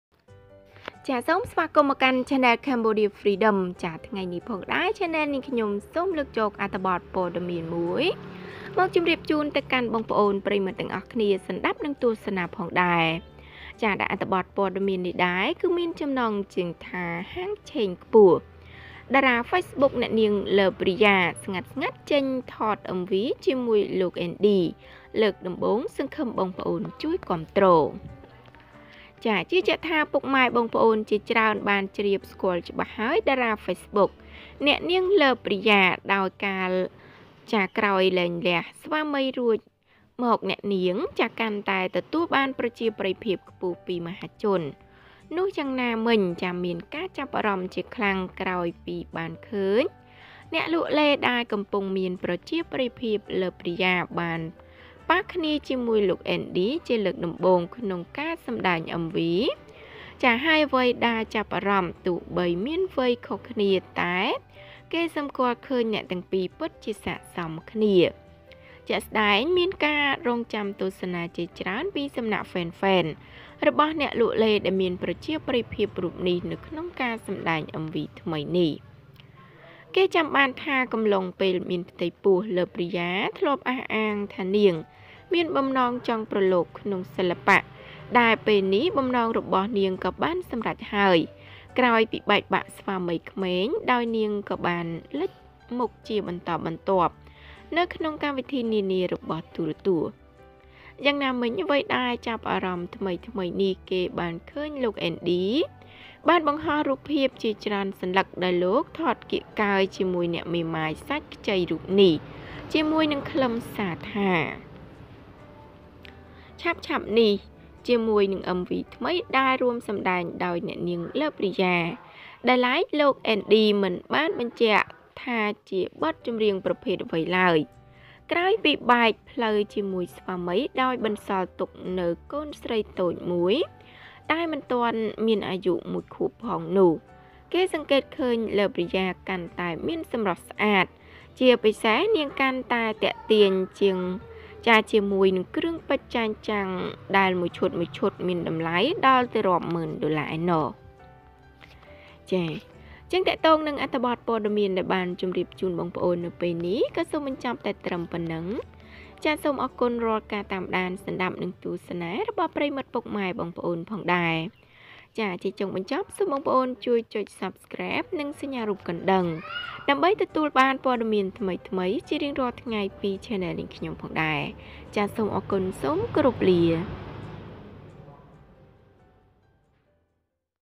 មានហាងឆេងខ្ពស់! តារាហ្វេសប៊ុកអ្នកនាង Love Riya ស្ងាត់ៗចេញថត MV ថ្មីជាមួយលោកអេនឌី លើកដំបូងសង្ឃឹម
Odds are high!  Facebook star Love Riya quietly released a new MV with Andy Hope

សូមស្វាគមន៍មកកាន់បណ្តាញព័ត៌មានសូមកុំភ្លេចចុះឈ្មោះជាវប៉ុស្តិ៍របស់ខ្ញុំសូមអរគុណ!

Cambodia Freedom

សូមអរគុណយ៉ាងជ្រាវជ្រៅវ
Thank You So much !